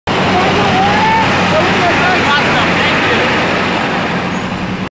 Downtown occupied with protesters
It appears there were several groups protesting today, although the majority that I saw were protesting in favor of racial reform and justice for citizens of color. However, I heard there were protests by people who were sick of protesters that were causing violence and looting, although that hasn't really been an issue other than the slave block that has been downtown for many years as a reminder that it hasn't been so long ago that our people thought it was okay to sell black men, women and children that were chained to cinder blocks. Their teeth and health inspected like animals and sold to the highest bidder, and I am surprised that this is the first time that block hasn't been defaced.